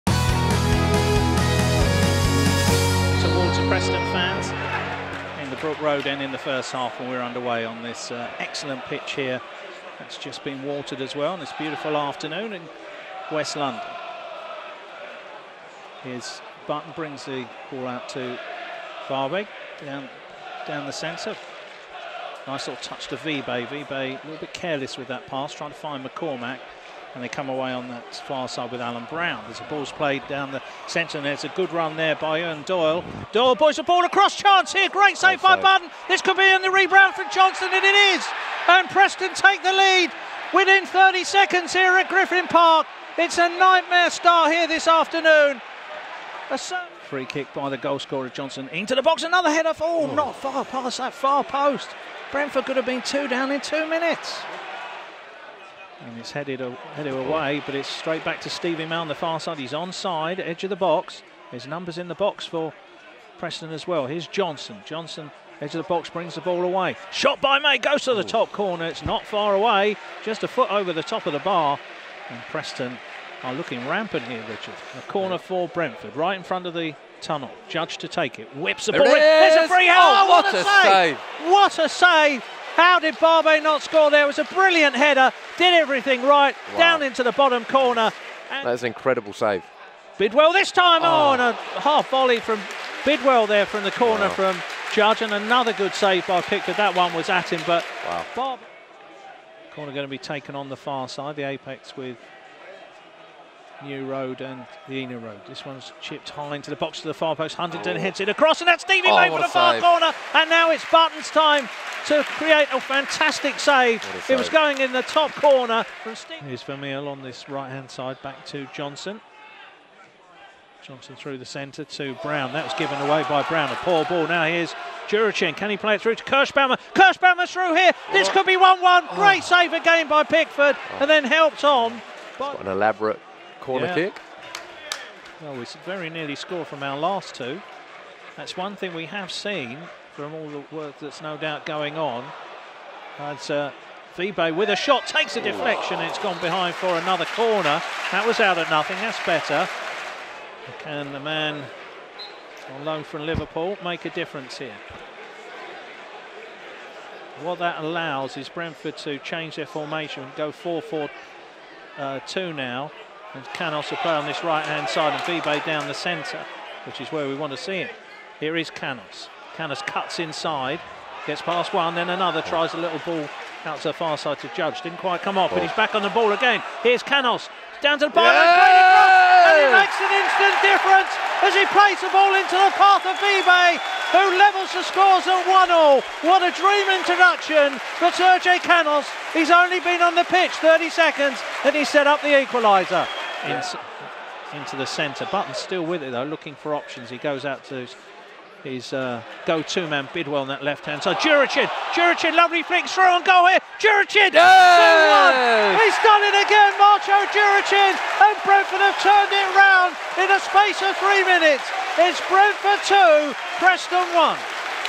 Match Highlights: Brentford 2-1 Preston North End
Brentford concede early, but it doesn't matter as they complete a turnaround through Lasse Vibe and Marco Djuricin.